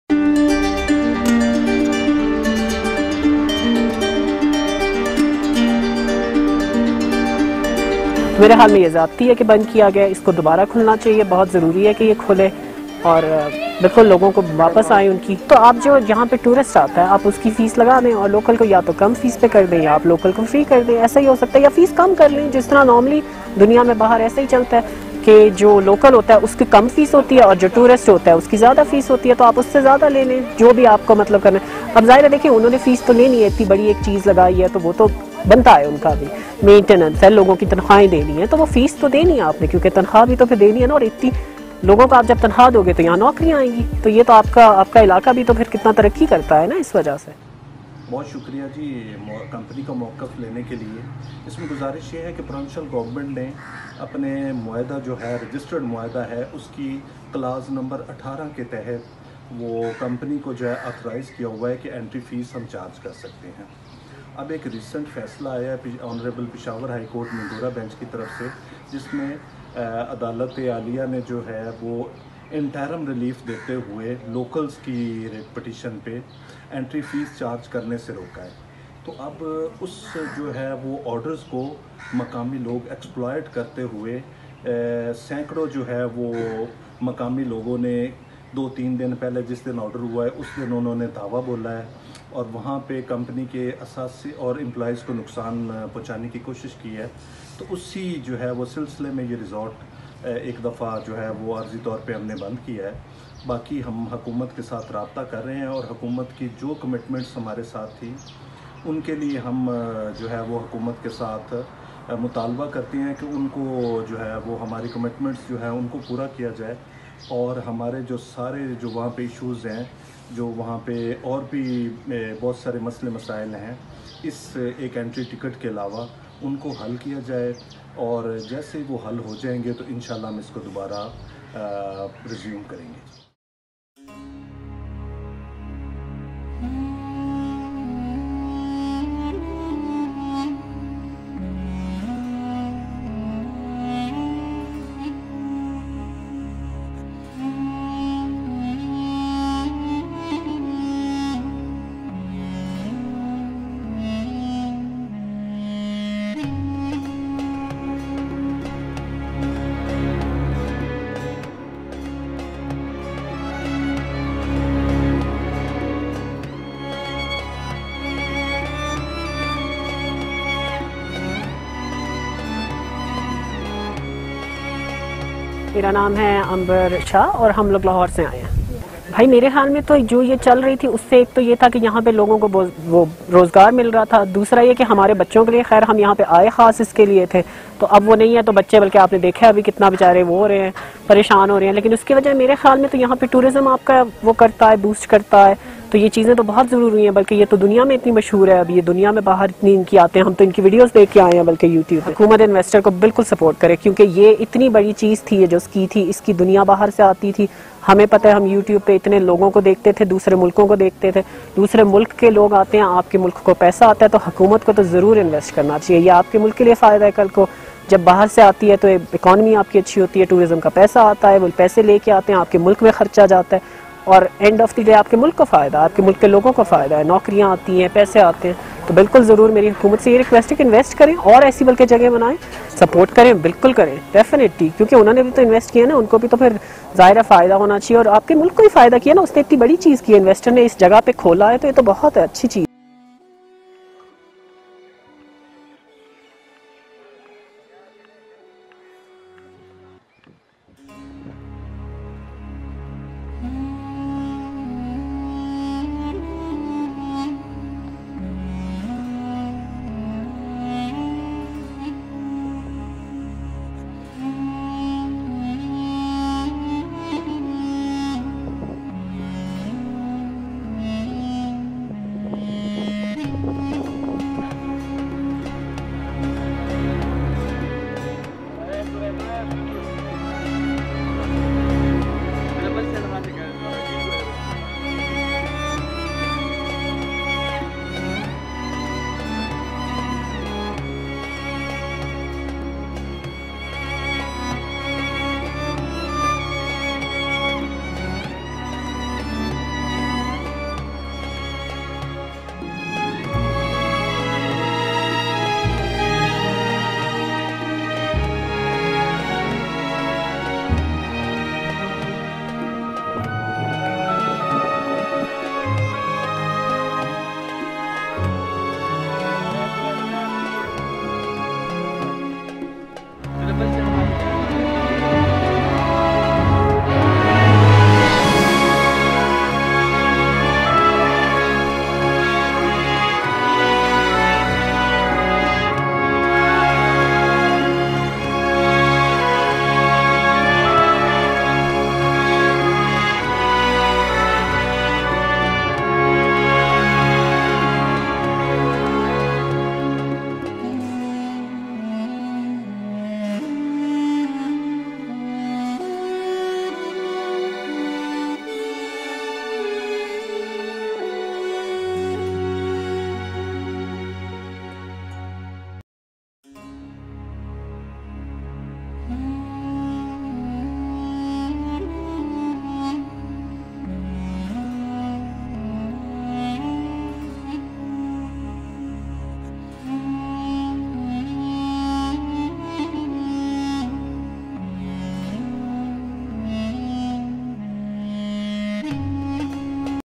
Malam Jabba Closed For Tourist l Fee Issue l Samson's Refused PHC Order l Swat l Kalam l PTI KP Govt
MalamJabba skiResort Closed down by Samson Pakistan Tourism in swat is sinking